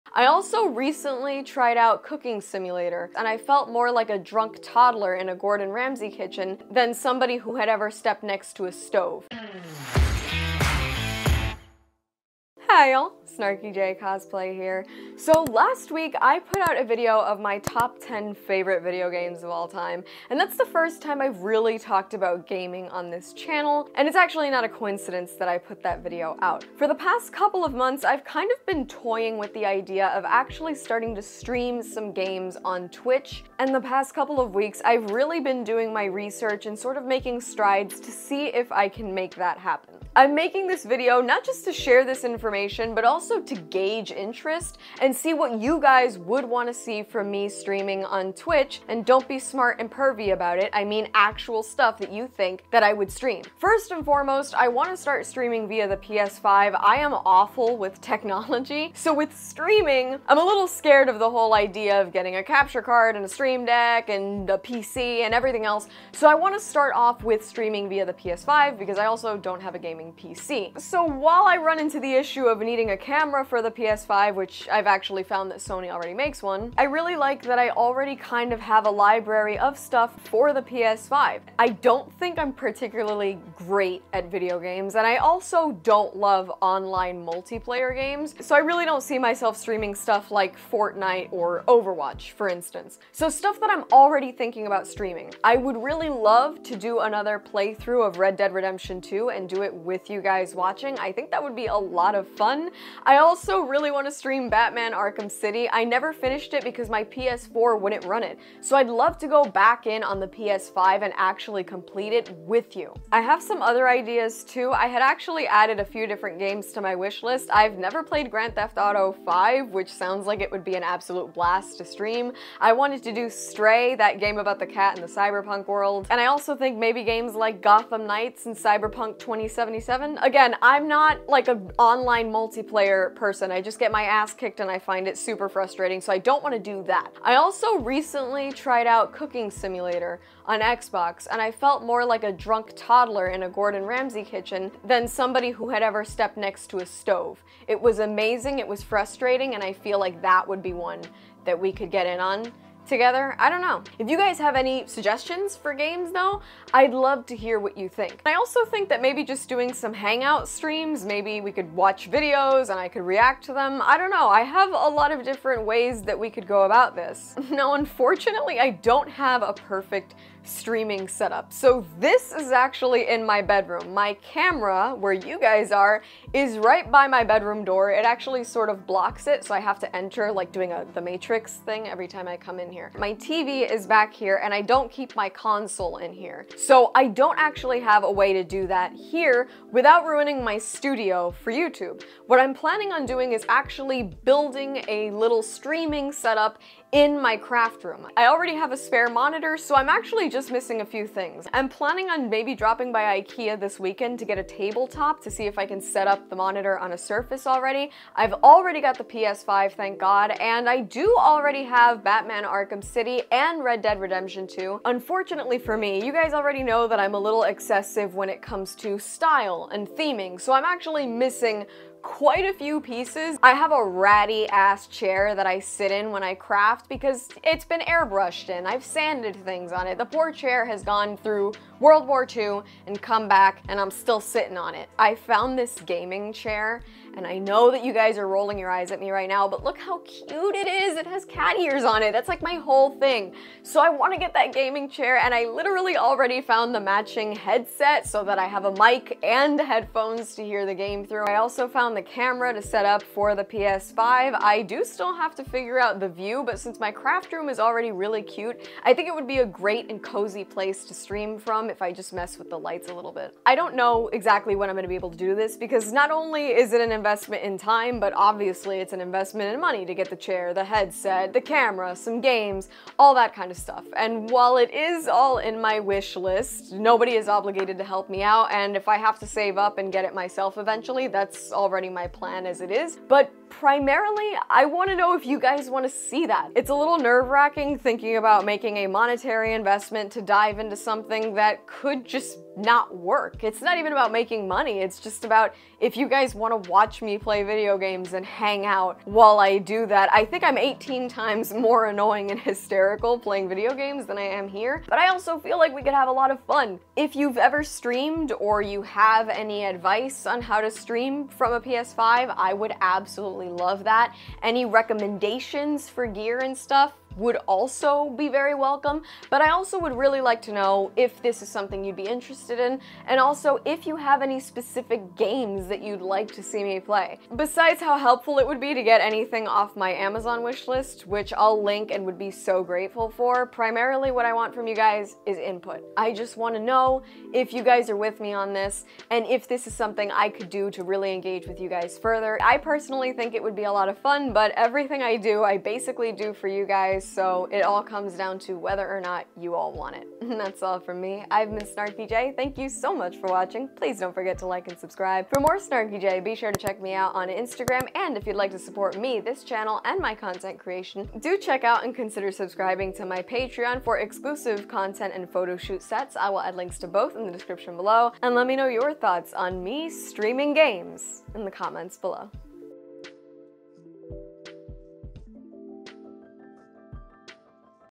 SHOULD I Start STREAMING PS5 Games on TWITCH!?
After a few years on YouTube, I'm thinking of finally starting to stream some games on Twitch! Here are my considerations, ideas, and what's holding me back...